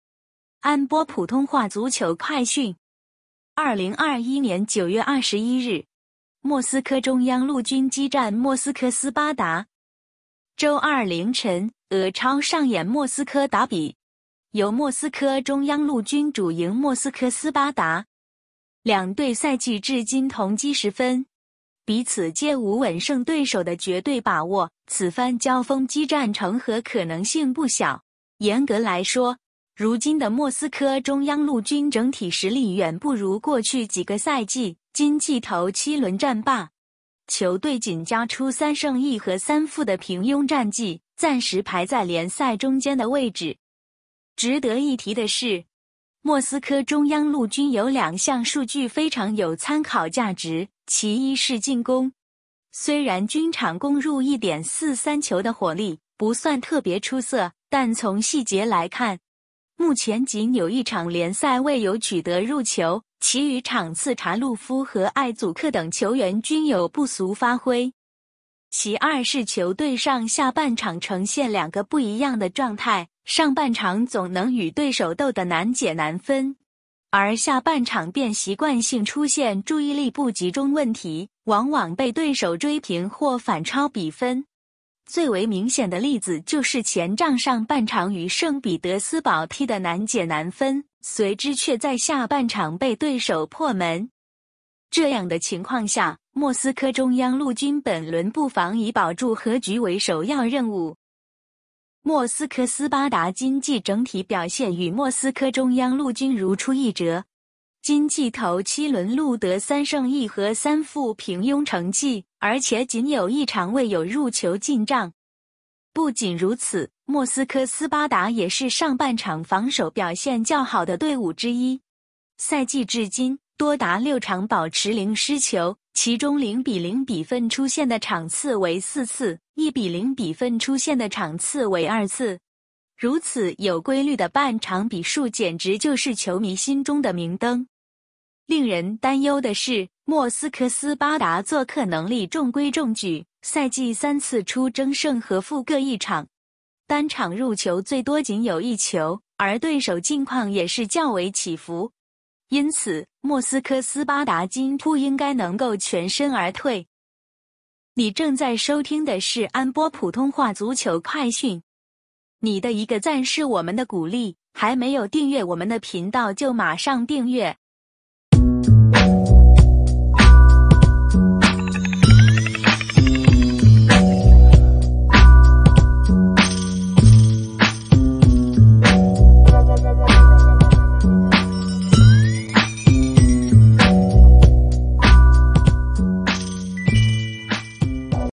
(普)【俄超-賽前新聞】2021-09-21 莫斯科中央陆军 VS 莫斯科斯巴达 | 莫斯科中央陆军激战莫斯科斯巴达
周二凌晨，俄超 上演莫斯科打比，由#莫斯科中央陆军 主迎#莫斯科斯巴达 。两队赛季至今同积10分，彼此皆无稳胜对手的绝对把握，此番交锋激战成和可能性不小。

严格来说，如今的莫斯科中央陆军整体实力远不如过去几个赛季，今季头7轮战罢，球队仅交出3胜1和3负的平庸战绩，暂时排在联赛中间的位置。值得一提的是，莫斯科中央陆军有两项数据非常有参考价值，其一是进攻，虽然均场攻入1.43球的火力不算特别出色，但从细节来看，目前仅有1场联赛未有取得入球，其馀场次查路夫和艾祖克等球员均有不俗发挥。其二是球队上下半场呈现两个不一样的状态，上半场总能与对手斗得难解难分；而下半场便习惯性出现注意力不集中问题，往往被对手追平或反超比分，最为明显的例子就是前仗上半场与圣彼德斯堡踢得难解难分，随之却在下半场被对手破门。这样的情况下，莫斯科中央陆军本轮不妨以保住和局为首要任务。

莫斯科斯巴达今季整体表现与莫斯科中央陆军如出一辙，今季头7轮录得3胜1和3负平庸成绩，而且仅有1场未有入球进账。不仅如此，莫斯科斯巴达也是上半场防守表现较好的队伍之一，赛季至今，多达6场保持零失球，其中0比0比分出现的场次为4次，1比0比分出现的场次为2次；如此有规律的半场比数简直就是球迷心中的明灯。令人担忧的是，莫斯科斯巴达作客能力中规中矩，赛季3次出征胜和负各1场，单场入球最多仅有1球，而对手近况也是较为起伏，因此，莫斯科斯巴达今铺应该能够全身而退。